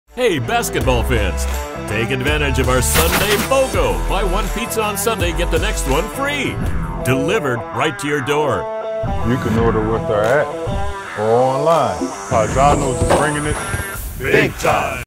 Paisano’s Pizza Goes BIG TIME With Markieff Morris - ESB Advertising
This is the 3rd commercial in the Paisano’s Pizza BOGO series. ESB Advertising and the Paisano’s Pizza team partnered with a DC Washington Wizard player for their latest commercial. In the spot featuring Markieff Morris, fans are encouraged to take advantage of Paisano’s ever so popular BOGO deal! On Sunday’s only, buy one pizza, get one free!